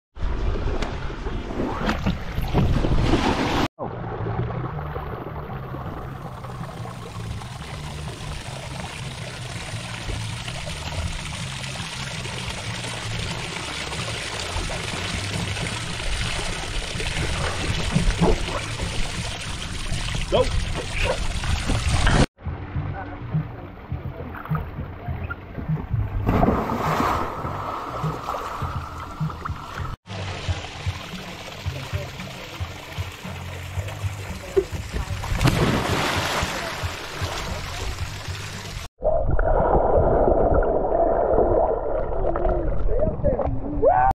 JUMPING THE SHARK OKAY JUMPING ON THE SHARK COMPILATION
Somebody wants to try jumping on the shark and nails it.